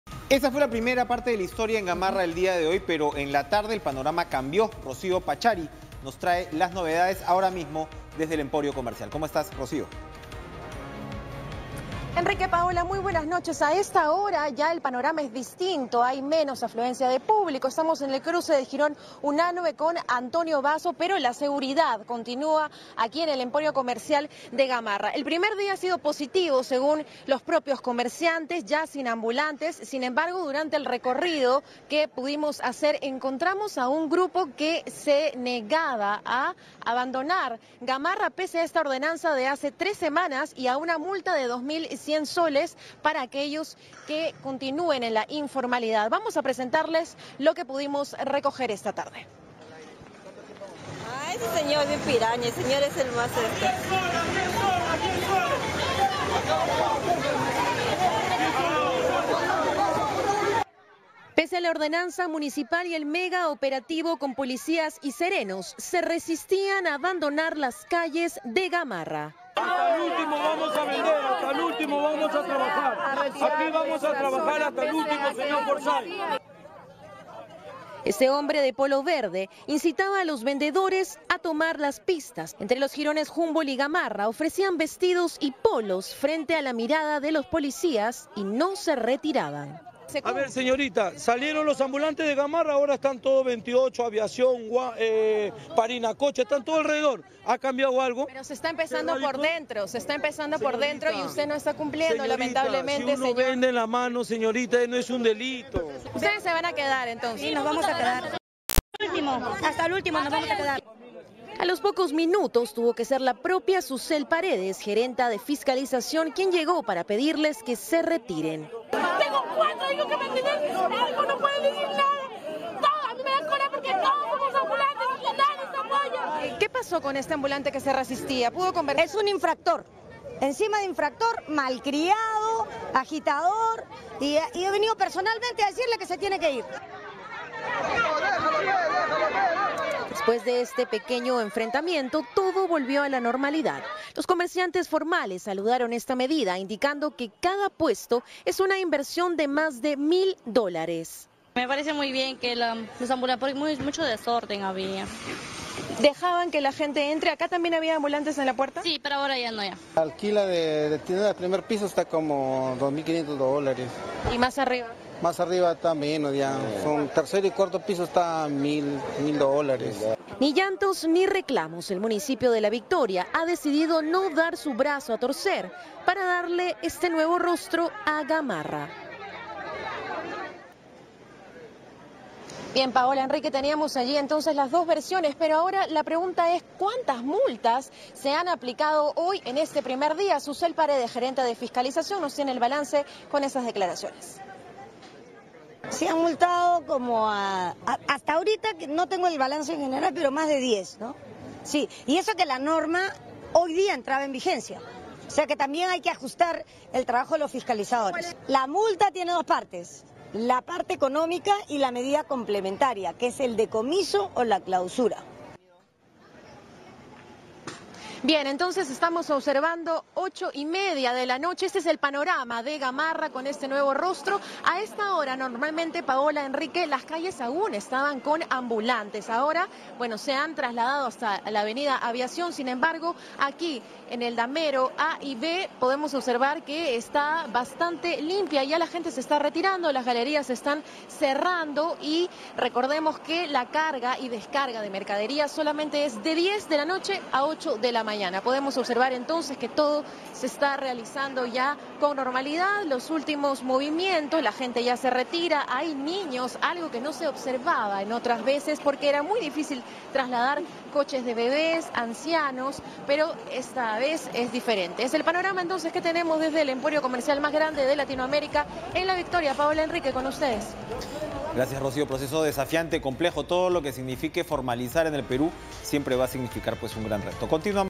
La Victoria: ambulantes ingresan a Gamarra pese a prohibición
Un grupo de ambulantes se rehusaron a salir de las pistas de Gamarra pese a la ordenanza municipal emitida hace tres semanas y a la multa que amonesta a quienes realicen el comercio ambulatorio.